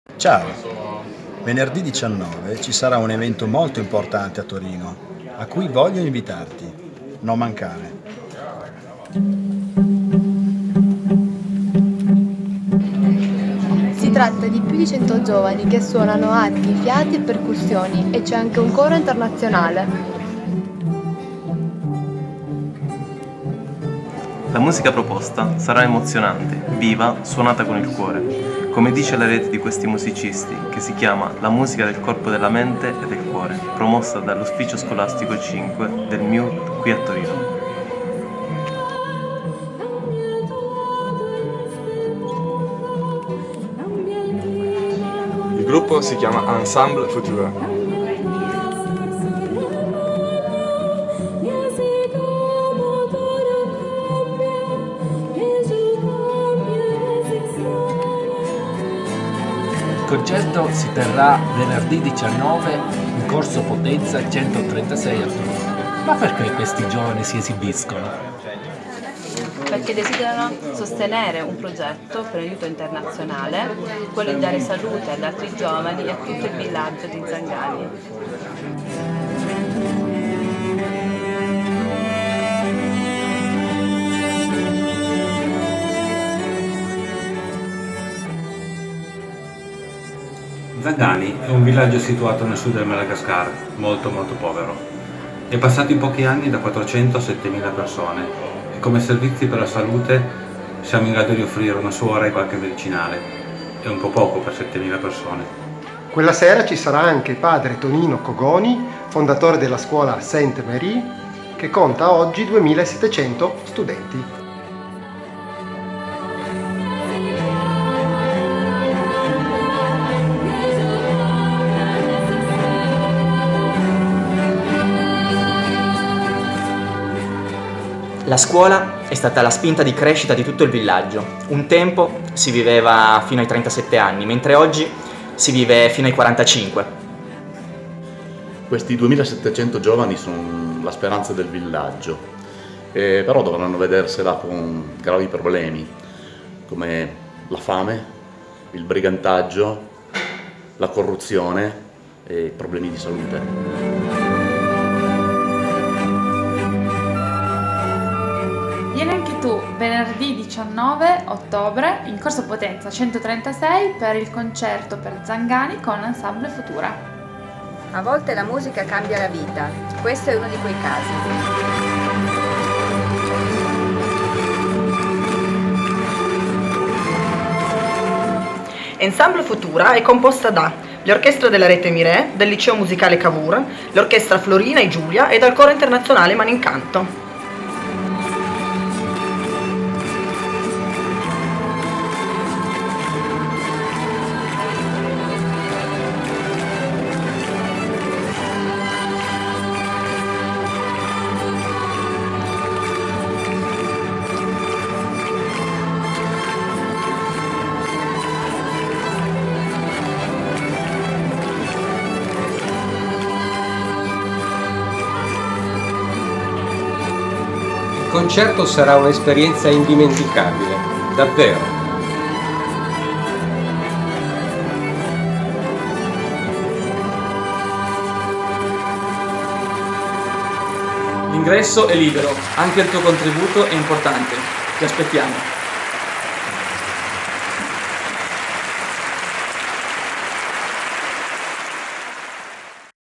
19 ottobre 2018 Concerto per Jangany ENSEMBLE FUTURA
Chiesa del Cottolengo
Corso Potenza 136 Torino
19 ottobre 2018 ore 21
Concerto per progetto di aiuto sanitario
amici di Jangany
a cura di ENSEMBLE FUTURA